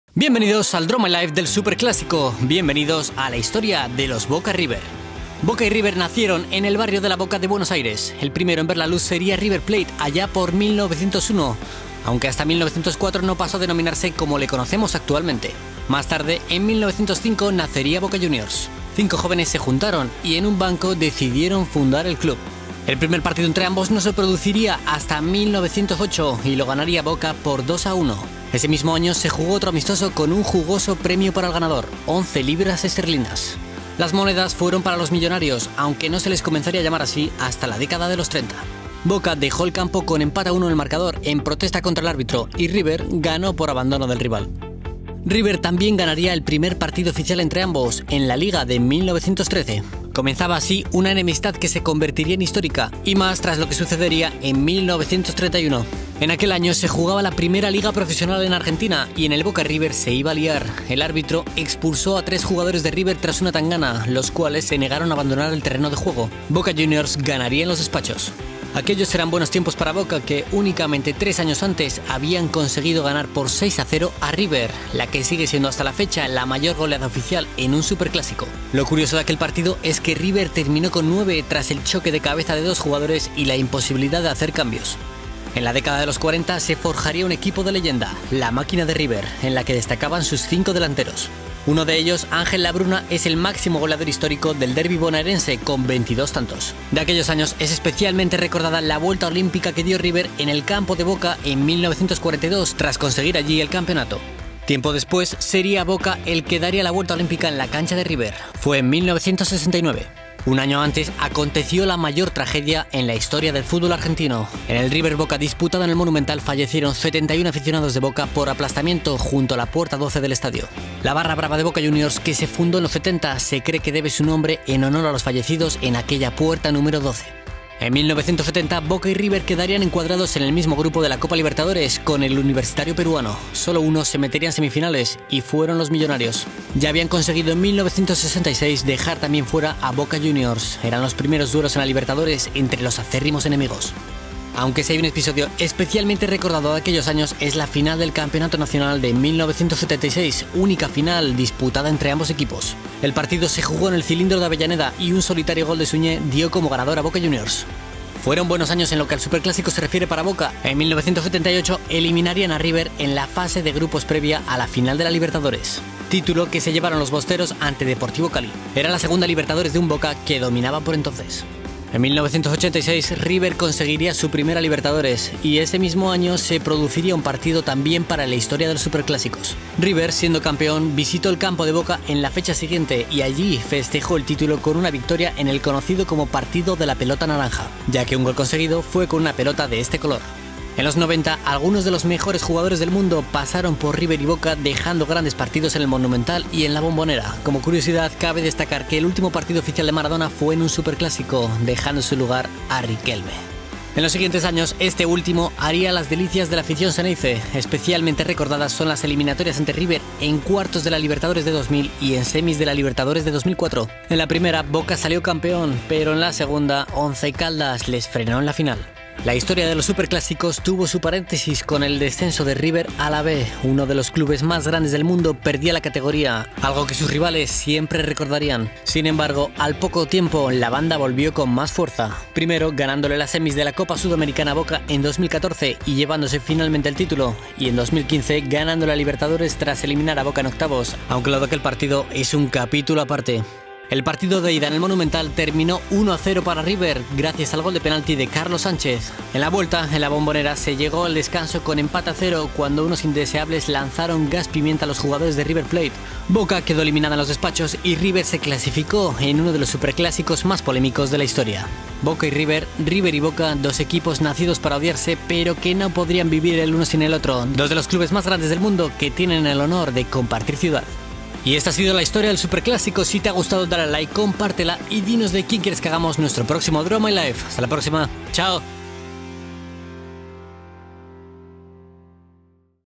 LA HISTORIA DE LOS BOCA-RIVER - Draw My Life
¡Bienvenidos al Draw My Life del Superclásico! Te contamos mediante divertidos dibujos la historia de enemistad entre Boca Junios y River Plate, dos de los clubes más grandes del mundo.